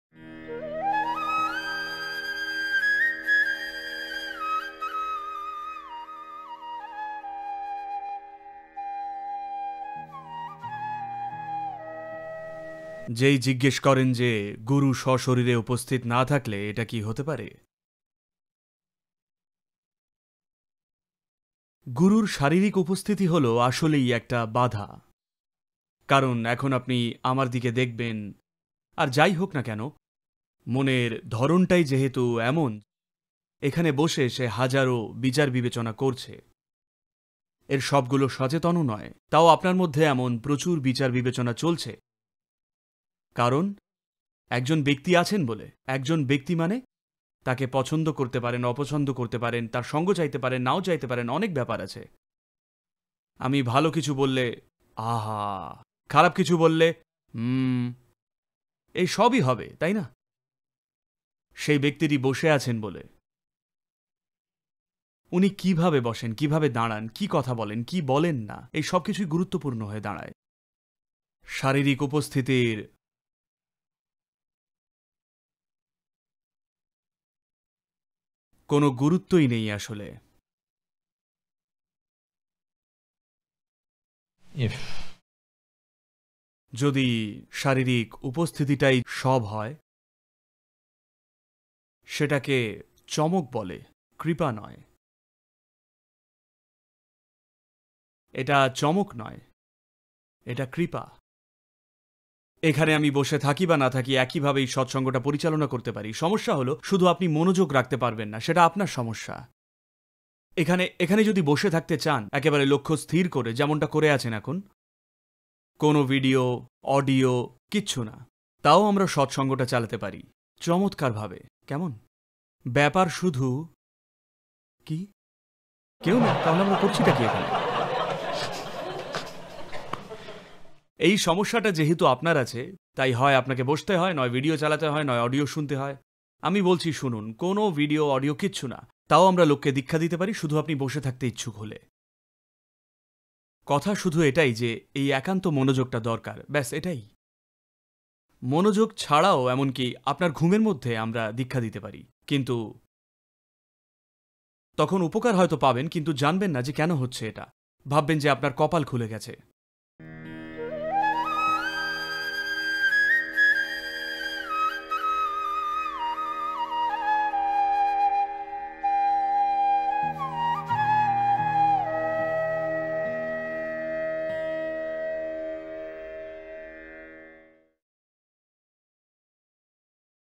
গুরুর শারীরিক উপস্থিতি কি আদৌ দরকার? | Physical Presence of Guru not Required
গুরুর সান্নিধ্য ছাড়া যে আধ্যাত্মিক পথে চলা যায় না এ আমরা সবাই জানি। তার মানে কি গুরুকে সশরীরে উপস্থিত থাকতে হবে? সামনাসামনি দীক্ষা দিতে হবে? নাকি দীক্ষিত হতে গেলে গুরুর শারীরিক উপস্থিতির কোনও দরকার নেই? আসন্ন গুরুপূর্ণিমা উপলক্ষে সদগুরুবাংলা 'র নিবেদিত এই বিশেষ ভিডিওটিতে আসুন দেখে নিই কী বলছেন সদগুরু...